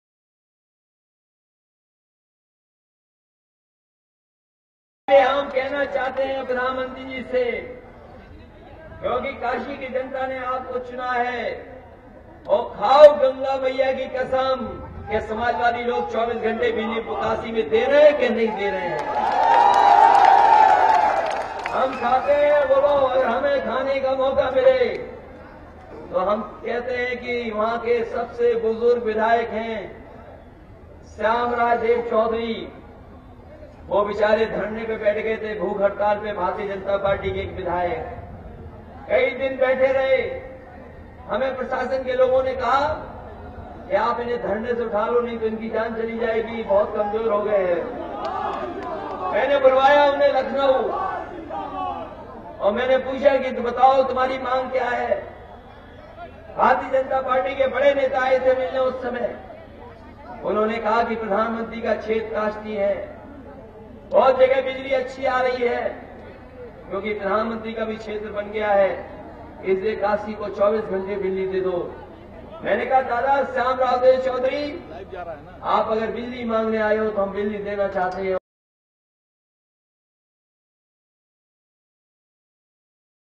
Urge PM Modi to see developmental work in his constituency Kashi : Akhilesh - ANI
Amethi (Uttar Pradesh), Feb 20 (ANI): Uttar Pradesh Chief Minister Akhilesh Yadav while addressing a rally in U.P.'s Amethi called out and said to measure the work done by Samajwadi Party (SP) during its tenure, PM Modi must visit his constituency Kashi where electricity has been provided 24*7.